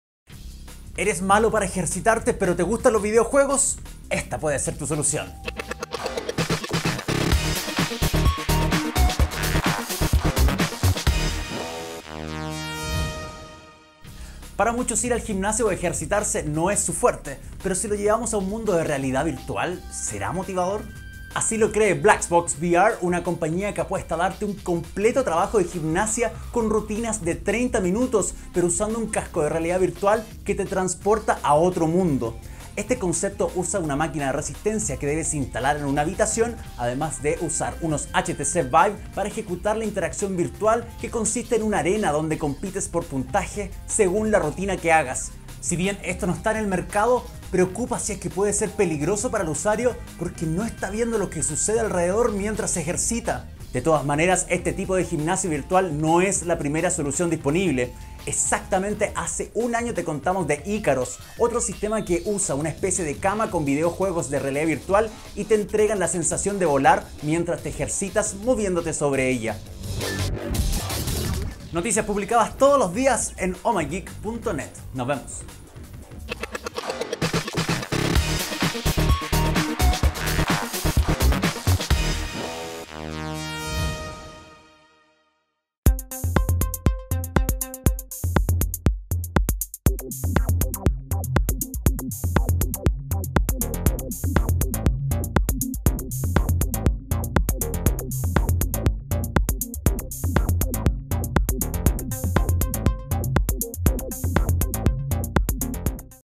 ¿No te gusta ir al gimnasio? 💪🏻 Deberías probar el ejercicio a través de realidad virtual
Black Box VR es el nombre de una plataforma -en concepto- que fue presentado en la pasada CES 2018 como una solución para motivar más el ejercitarte a través de la gamificación y la realidad virtual.